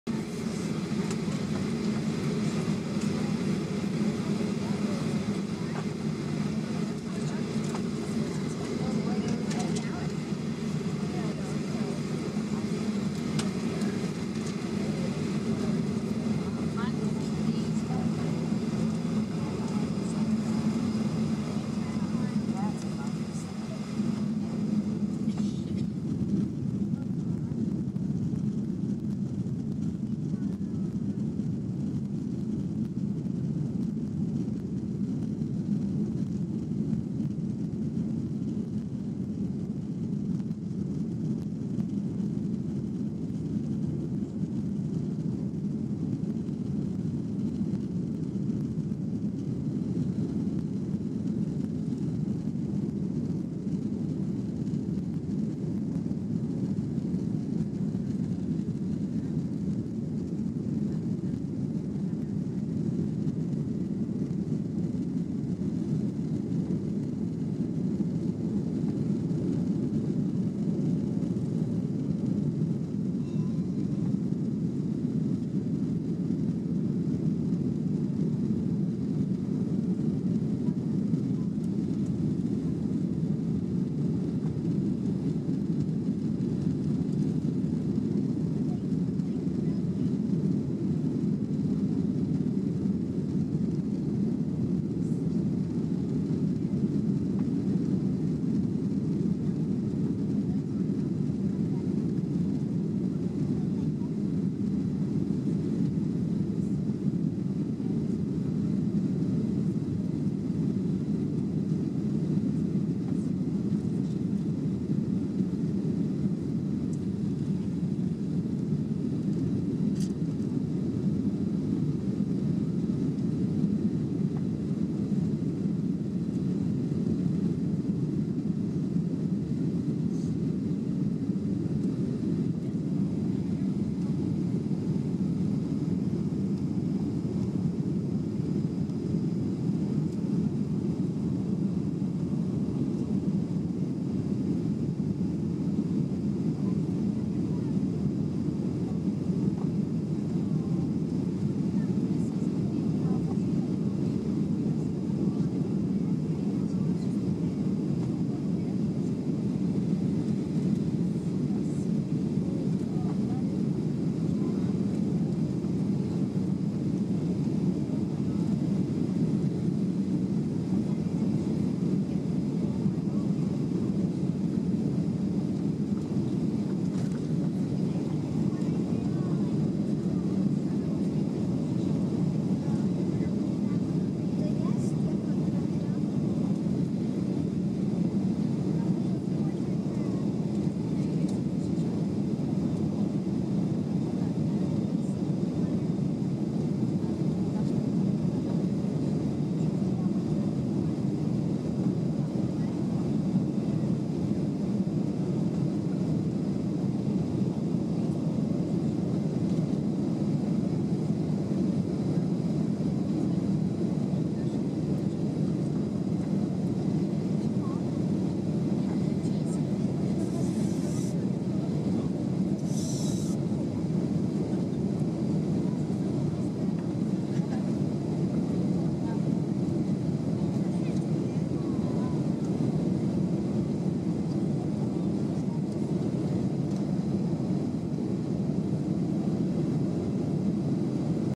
American Airlines B737-800 Take-Off Ft. Myers
American Airlines flight AA1284 leaves Southwest Florida Intl' Airport via runway 06. The flight left for Dallas Ft. Worth, Texas. The flight was operated by a Boeing B737-800 registered as N864NN.
Date: 8-2-14
Aircraft: B737-832
Registration: N864NN
Departure: 14:25 lcl
Arrival: 15:44 lcl
Seat: 29F